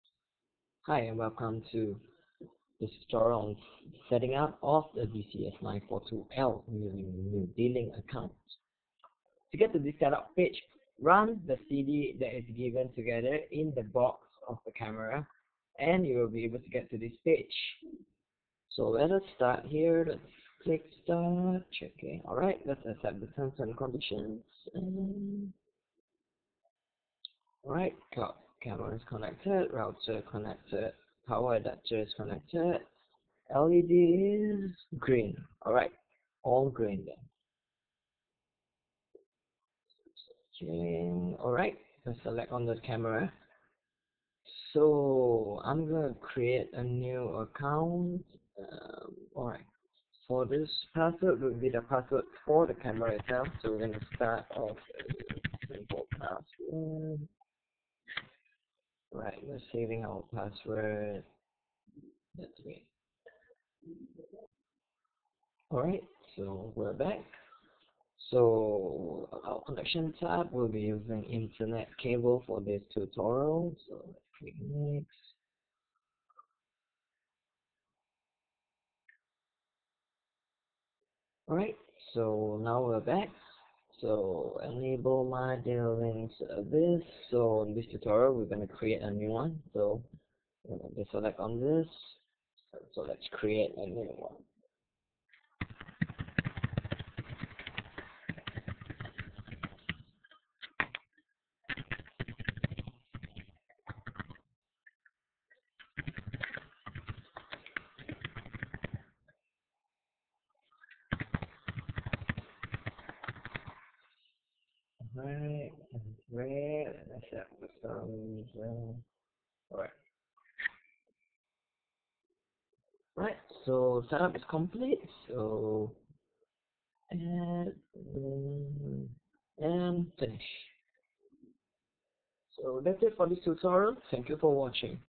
Cloud How-To : How to setup a new D-Link cloud IP Camera and access it from
Watch this video on how to setup your new D-Link cloud IP Camera and access it

This guide is good for all D-Link cloud IP Camera such as DCS-930L. DCS-942L, DCS-5222L, DCS-5020L, DCS-6010L, DCS-7010L with all the "L" series.

Is this video useful to you? Would you like to see more of such video? We love to hear from you.